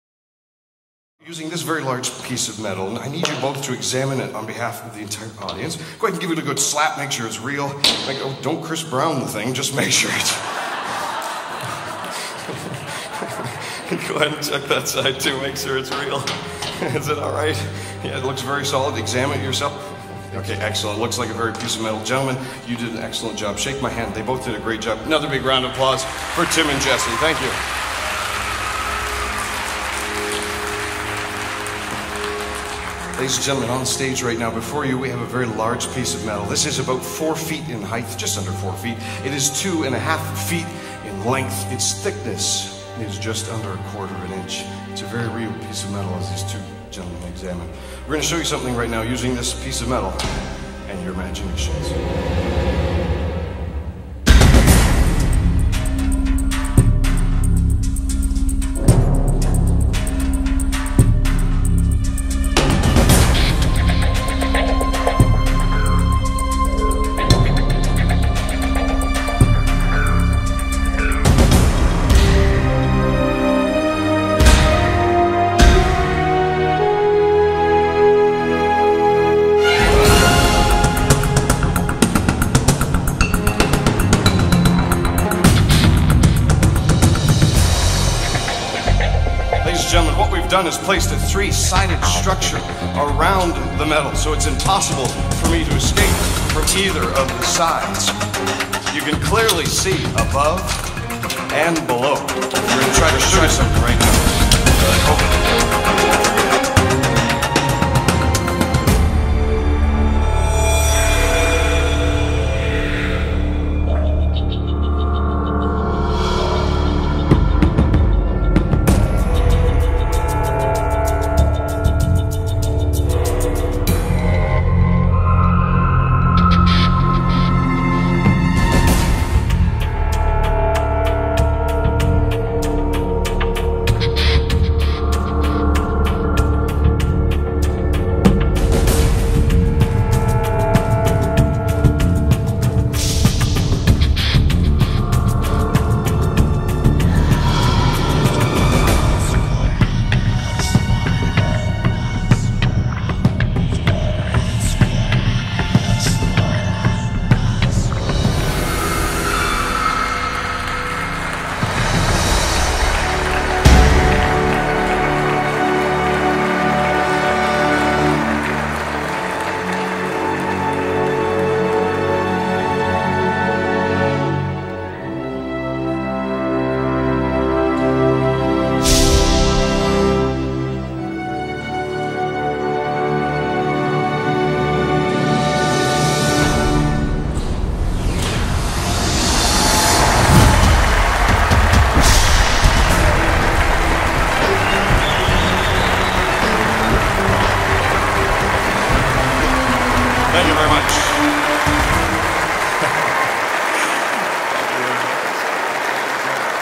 Illusionist walks THRU steel
This is a solid-thru-solid illusion with a magician and a large piece of steel.  Magician Ryan Joyce presents his take on the classic of magic, walking through a solid object.  Watch this Canadian stage illusionist walk through a solid metal sheet.

This illusion was designed first performed by Ryan Joyce and built at Tim Clothier's Illusion Projects in Las Vegas.  The effects official title is "Rite of Passage" Here's the effect:

THE EFFECT:
Two spectators are welcomed on stage to examine a large piece of metal.

The metal is secured between two posts and the magician stands behind the steel and fully enclosed.  The only way he can go is through the steel.  Portions of the metal sheet are covered and the magician's hand can be seen pushing the fabric spandex away from the metal.    The magician's arm fully penetrates the steel and Ryan is revealed to be halfway through the steel.

The illusion is revolved all the way around.

After centring back to the magician walks completely through the steel.

This concept and design were influenced by Timothy Drake, a fellow Canadian magician.

MAGIC'S HISTORY
Throughout history magicians have presented their own original presentations of a classic illusion, walking through a solid object.  Here are a few

Doug Henning walked through bricks, David Copperfield walked through steel and the Great Wall of China.  Similarly, Criss Angel walked through the glass and a metal gate.

Hope you like this version of Walking Through Solid Metal!